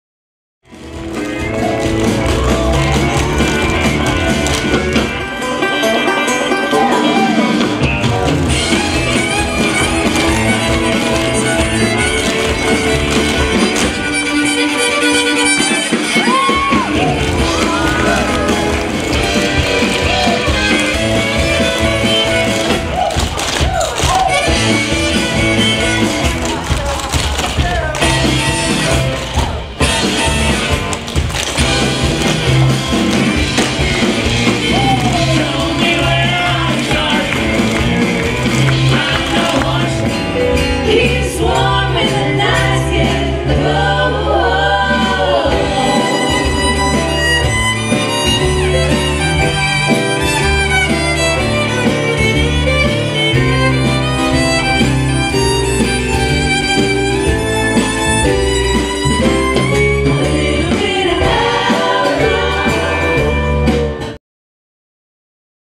Andrew Crowe - Fiddle - Medora Musical 2019
Brief fiddle/violin solos from the Medora Musical 2019, played by Andrew Crowe